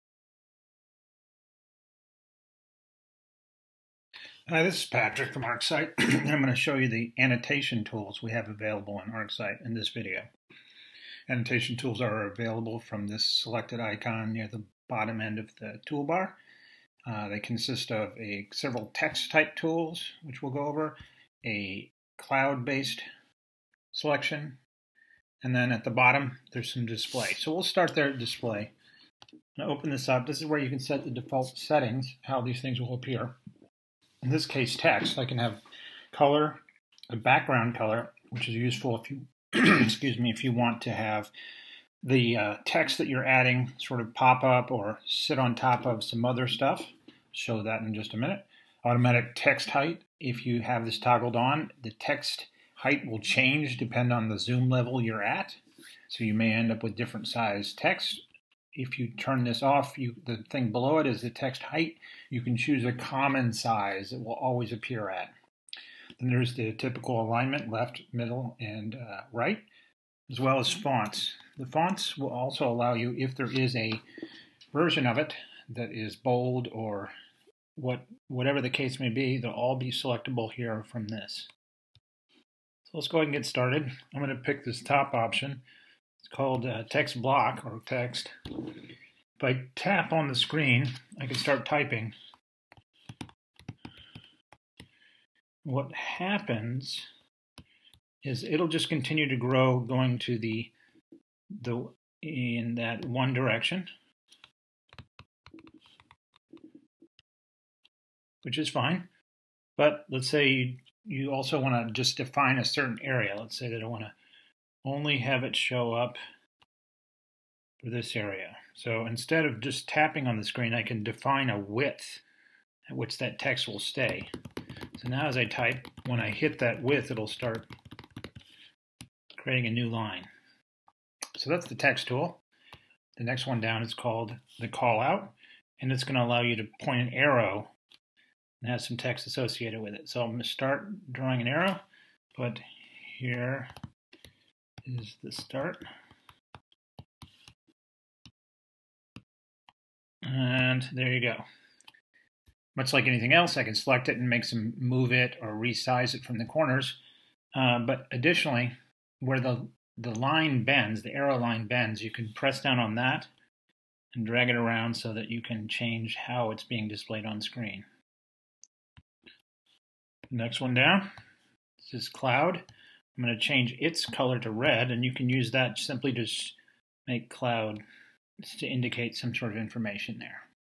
Annotate with Text and Cloud tools
Learn how to create text, use the cloud shape tool and how to set the display settings for both.